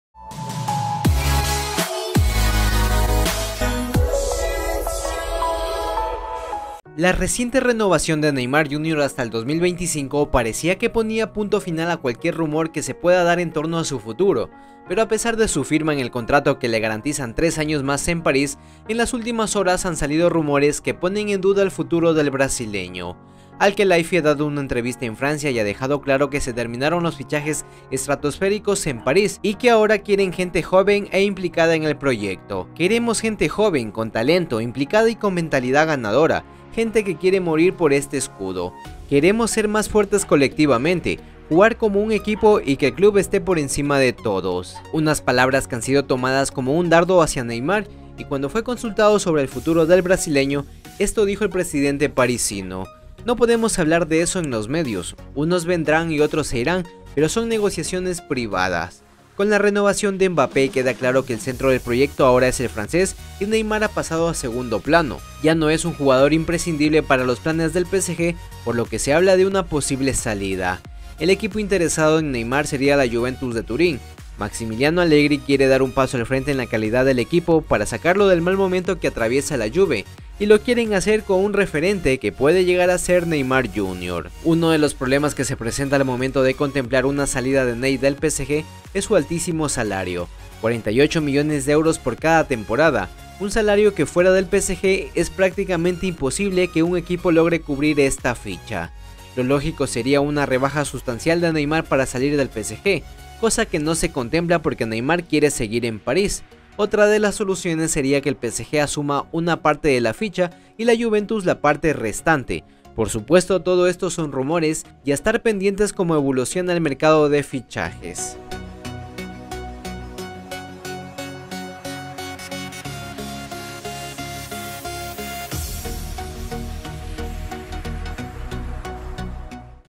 El PSG se PLANTEA la SALIDA de NEYMAR, YA NO es IMPRESCINDIBLE para el PROYECTO, JUVE INTERESADA
La Juventus estaría valorando la incorporación de Neymar. No es la primera vez pero parece que ahora hay más opciones. Massimiliano Allegri, técnico del equipo blanquinegro, ha pedido elevar el nivel del equipo y el brasileño podría hacerlo.

Además, la situación del delantero brasileño en el Paris Saint Germain no es sólo incierta sino que no es nada descartable que el equipo francés quiera prescindir de él.

Con Di Maria y Pogba también en la órbita del equipo italiano, está también sobre la mesa el regreso de Cristiano Ronaldo. Jorge Mendes habría propuesto al portugués que volviera a jugar en la Juventus, algo que hoy por hoy no es descartable, aunque tanto Neymar como Ronaldo tendrían que rebajar sensiblemente sus emolumentos.